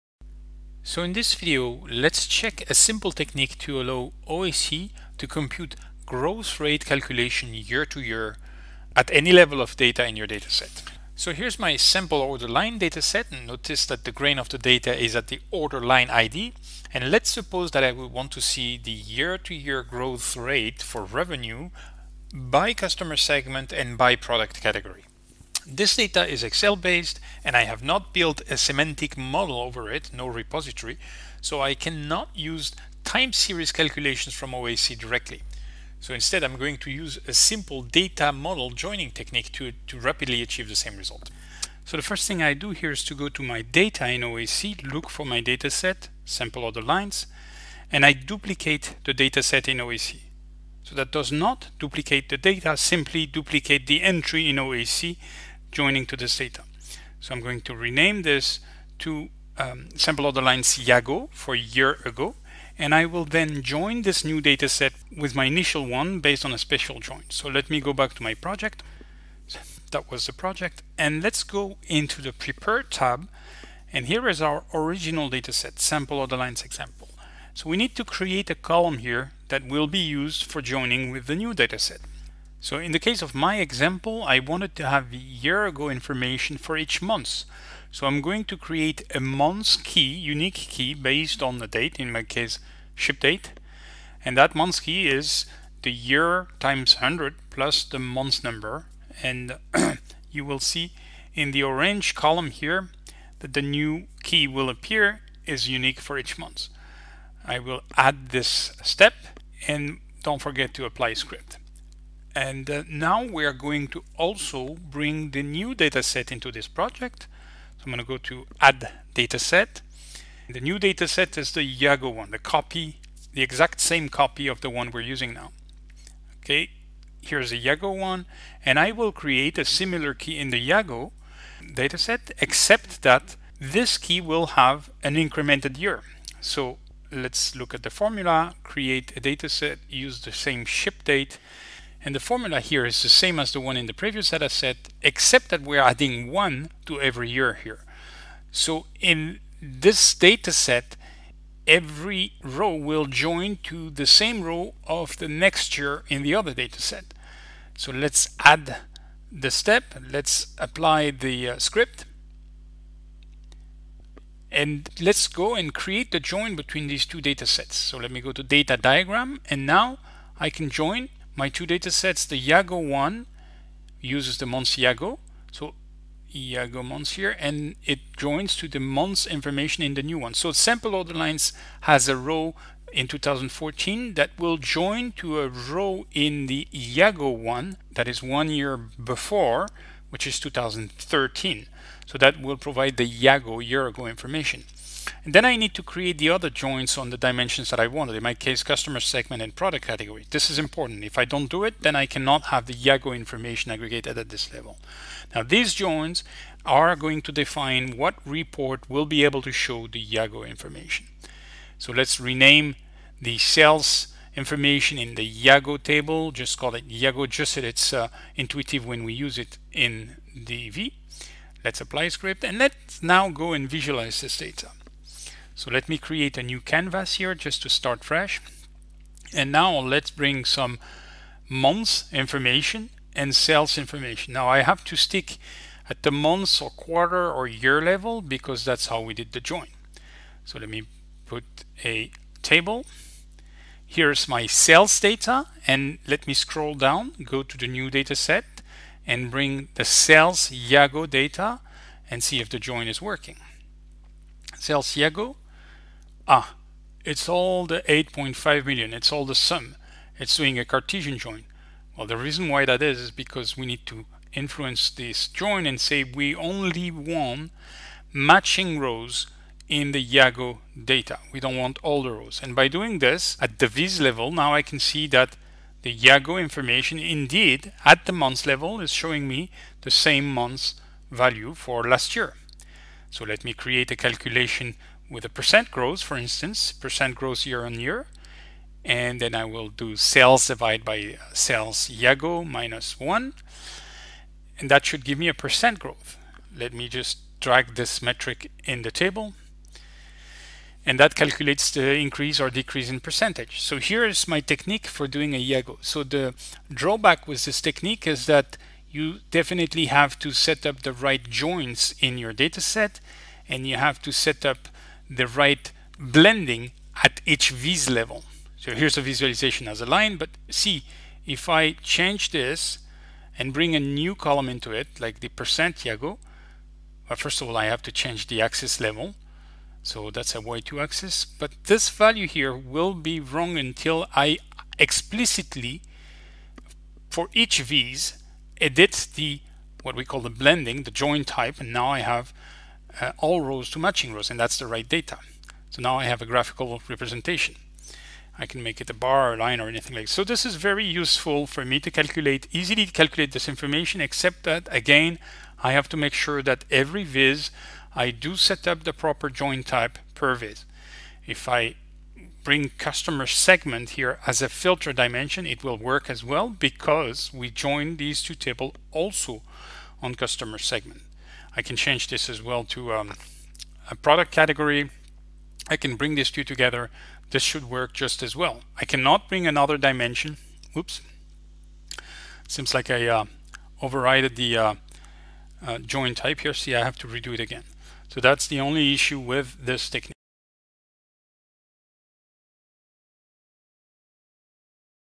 OAC Tips&Tricks : Period to Period Calculation in DV Without RPD Modeling
This video explains a simple technique to enable Period to Period calculation in Oracle DV without any modeling in the OAC semantic layer (no time hierarchy in the RPD).